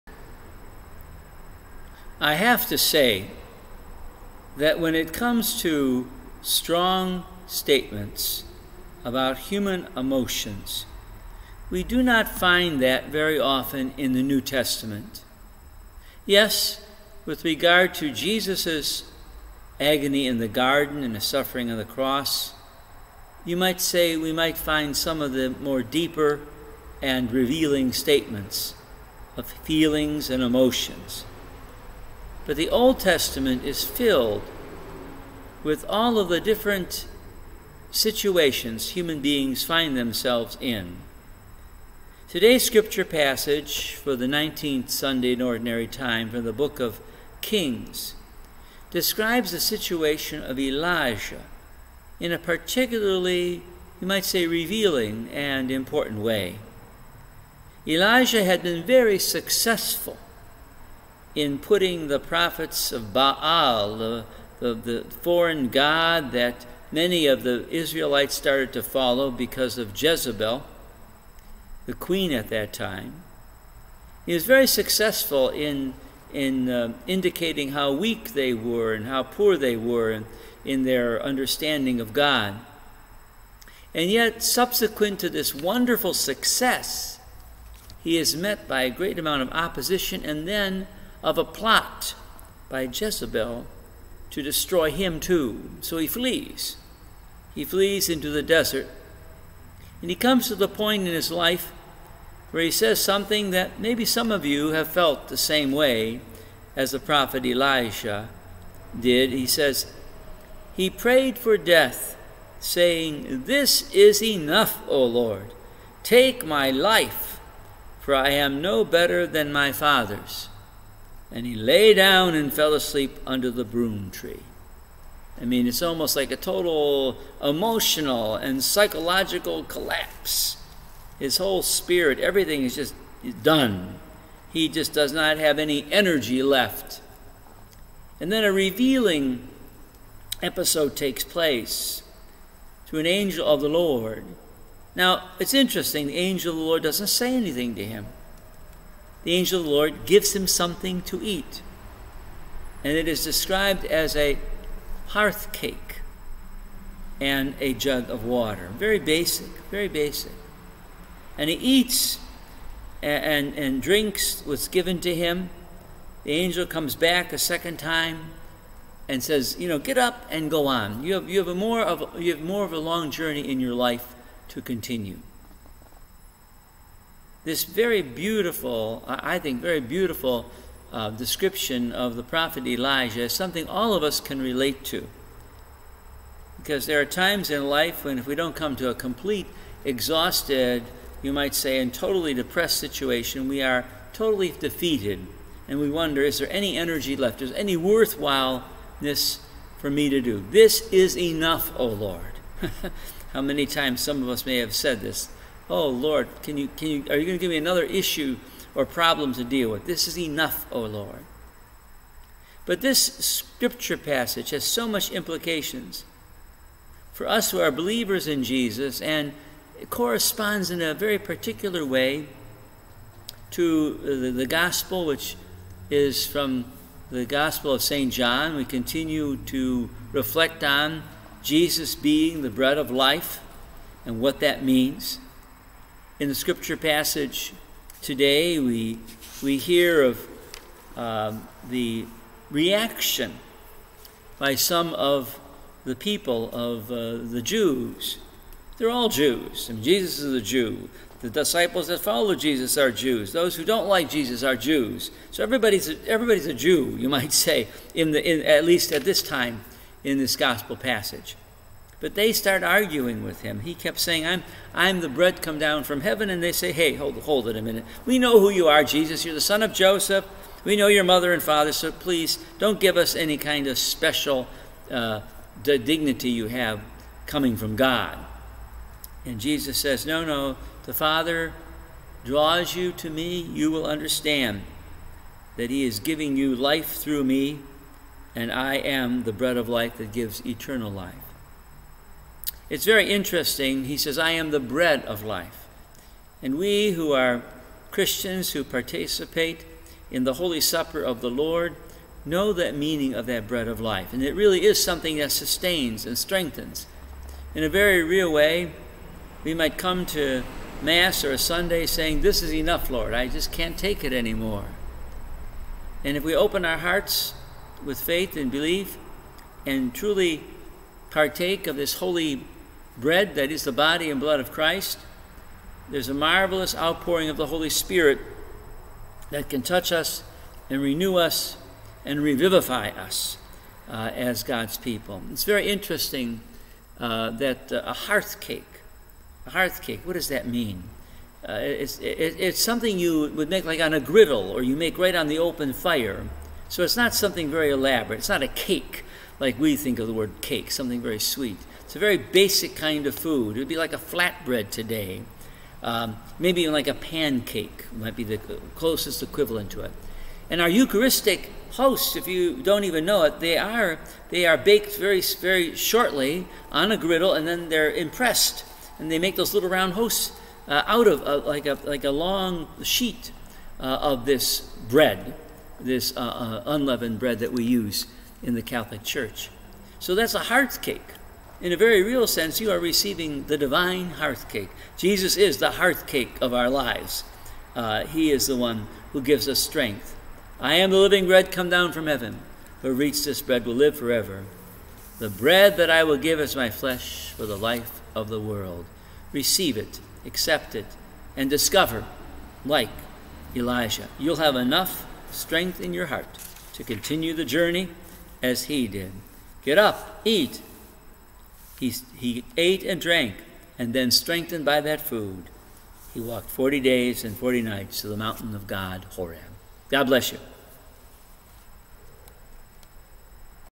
Heavenly Hearth Cake
What sustained Elijah in his deep depression is available to us in the Eucharist, the Bread of Life, Jesus!